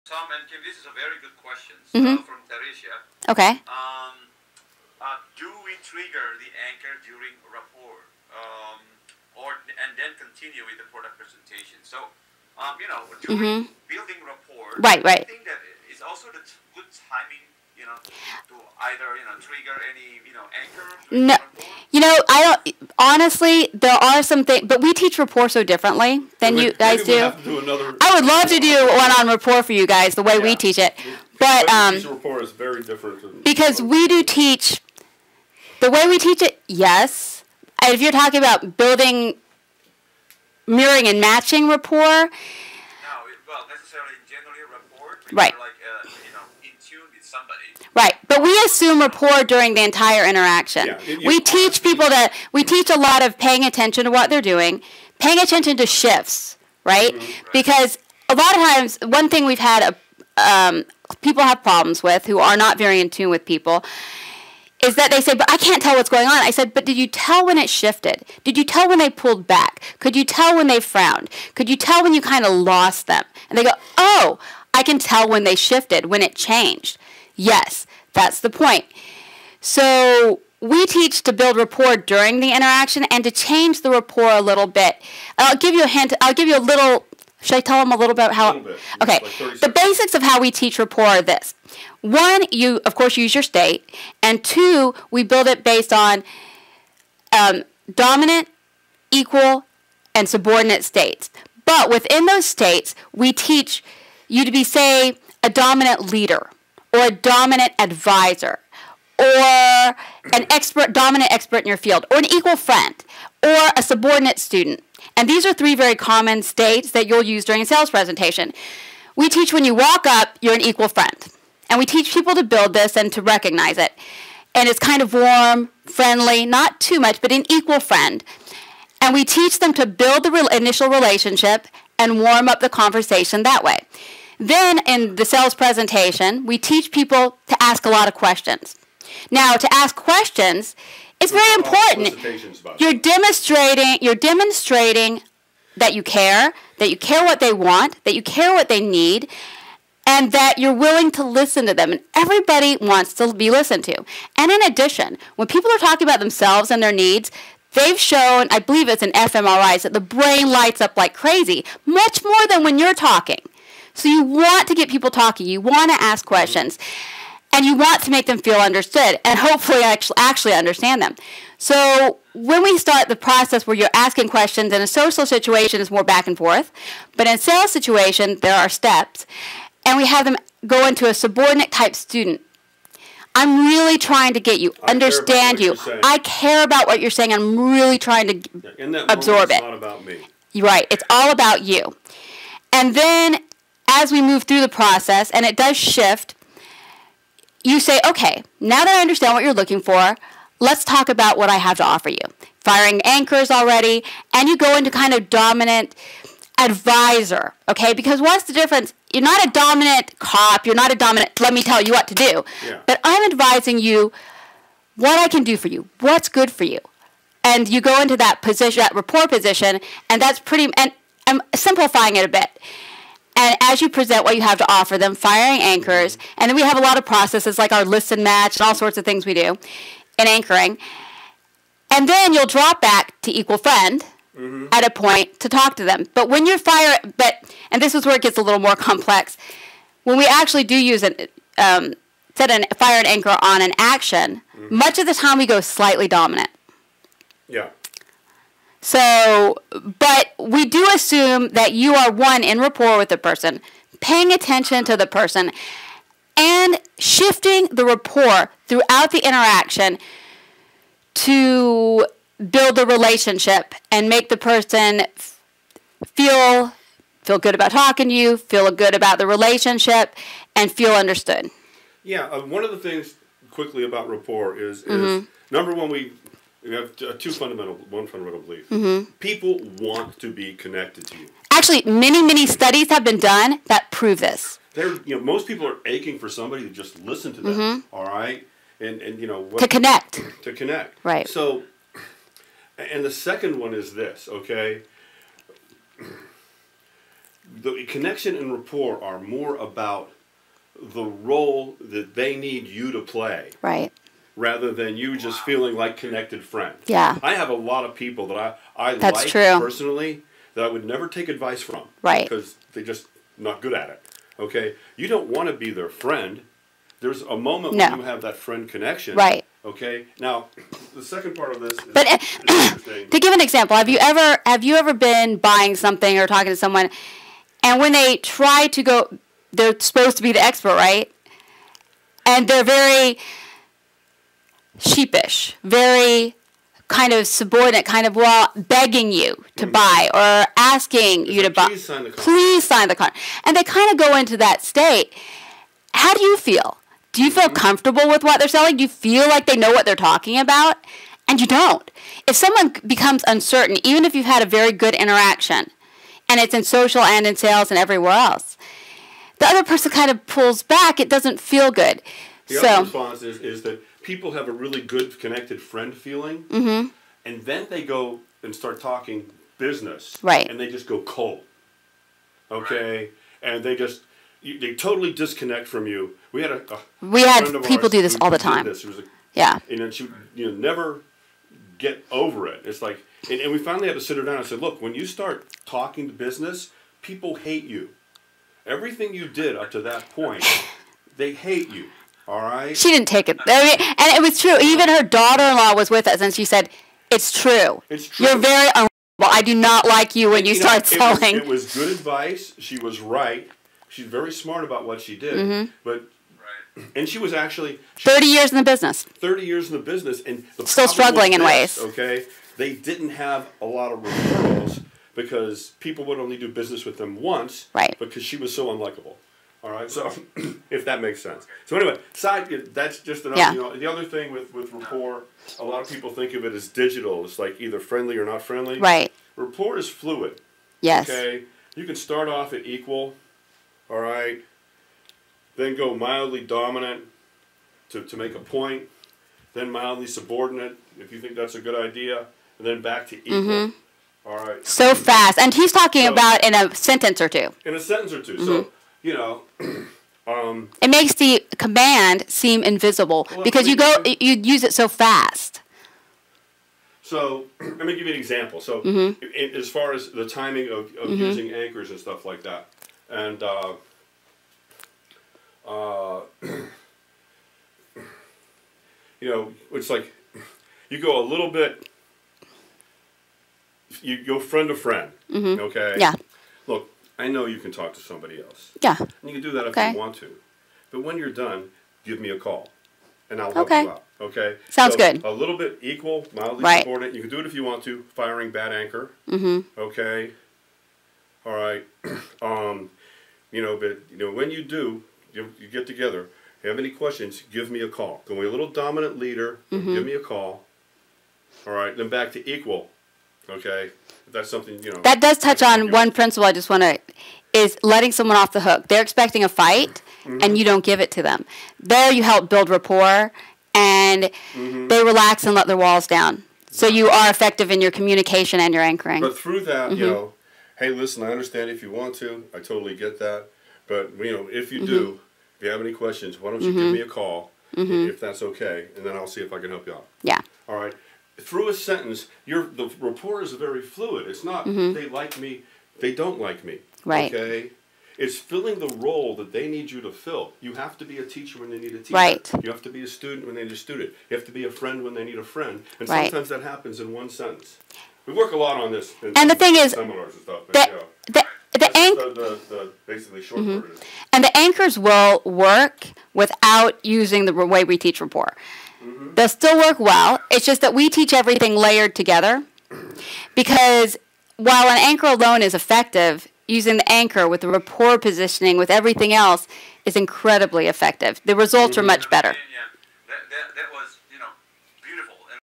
5 Rapport and using anchoring
How to use a combination of Rapport and Anchoring to the the best result in your sales, management or Influence

The Persuasion Codes Advanced Sales Success and Influence Skills